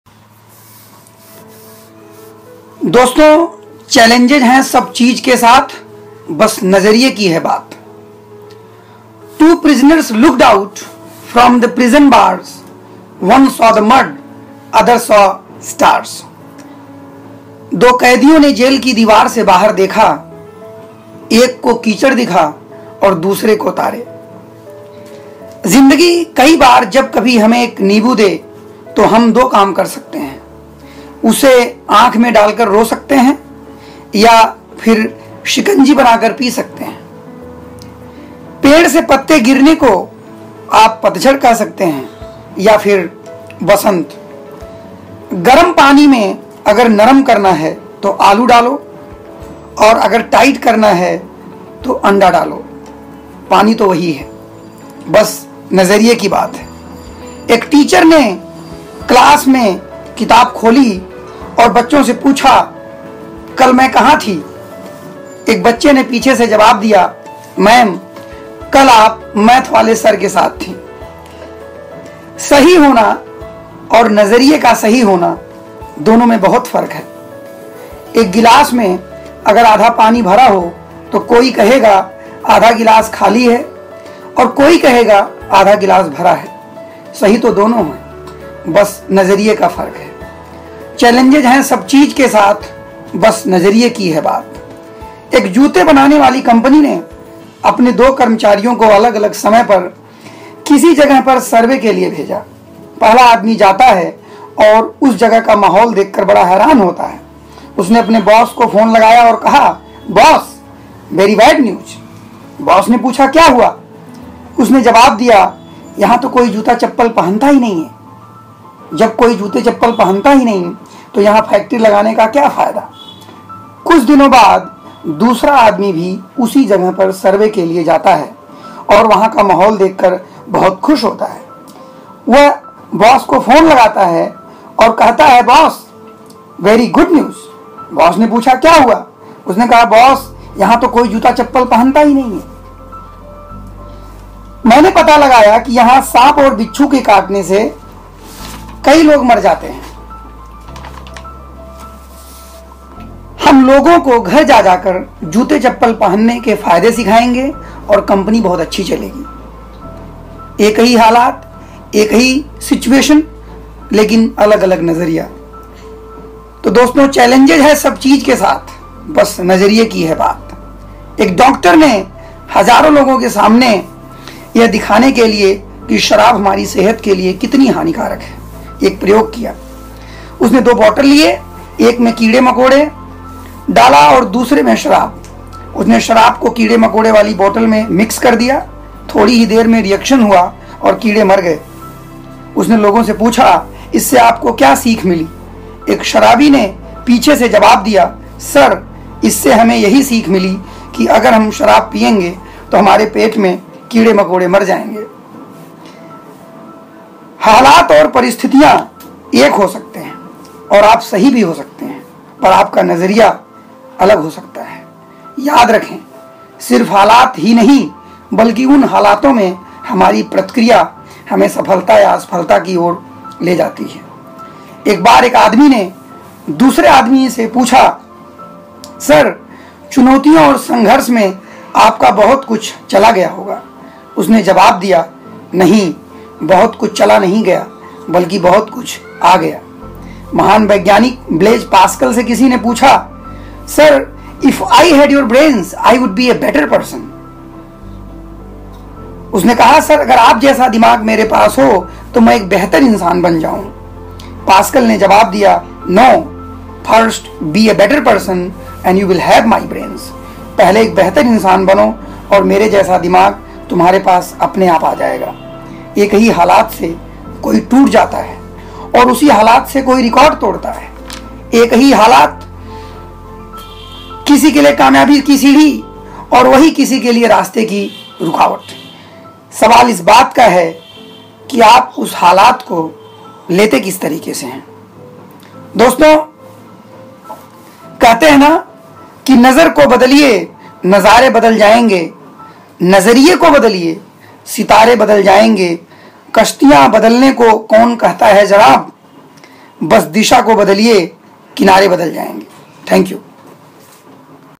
Attitude That Matters the Most (बस नज़रिए की है बात)
आप सही हो सकते है पर आपका नजरिया गलत हो सकता है ( Being right doesn't mean your attitude is right)